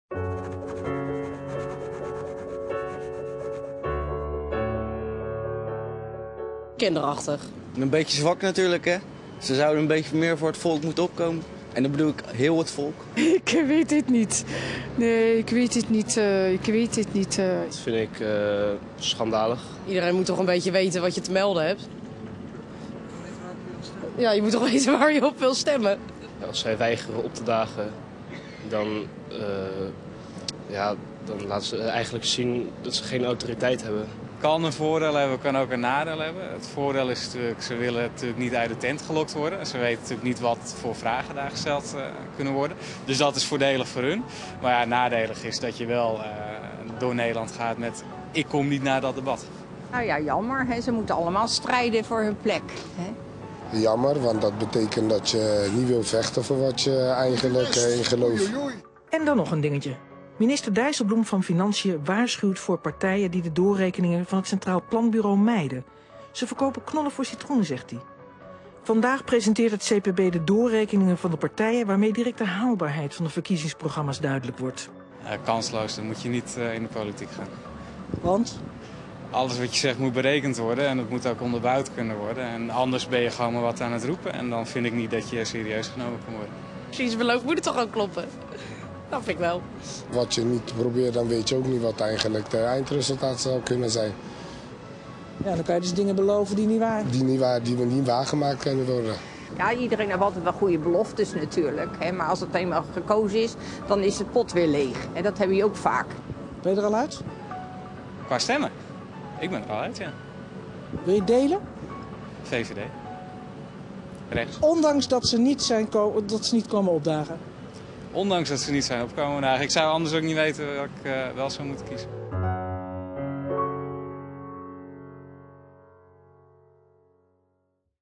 De Leidse Kooi: 'VVD en PVV vechten niet voor hun geloof'
In de serie Kiezers uit de Kooi, volgen we op weg naar de Tweede Kamer
verkiezingen ‘gewone’ mensen uit een ‘gewone’ wijk: de Leidse
Kooi. Vandaag de vraag: wat vind je ervan dat de VVD en de PVV
bedanken voor het lijsttrekkersdebat bij RTL omdat er meer dan vier
partijen uitgenodigd waren. 'Deze partijen vechten niet voor hun
geloof', zegt een kiezer. 'Ze laten eigenlijk zien dat ze geen
authoriteit hebben', zegt een andere man uit de Kooi. En de tweede
kwestie die de kiezers uit De Kooi voorgeschoteld krijgen is het
volgende. Minister Dijsselbloem van financien waarschuwt voor partijen
die de doorrekeningen van het Centraal Planbureau mijden. 'Ze verkopen
knollen voor citroenen', zegt hij. Vandaag presenteert het CPB de
doorrekeningen van de partijen, waarmee direct de haalbaarheid van de
verkiezingsprogramma’s duidelijk wordt. 'Alles wat gezegd wordt moet
berekend worden, anders ben je maar wat aan het roepen', zegt een
kiezer. Een andere man uit de wijk zegt: 'Dan worden er dingen beloofd
die niet waargemaakt kunnen worden.'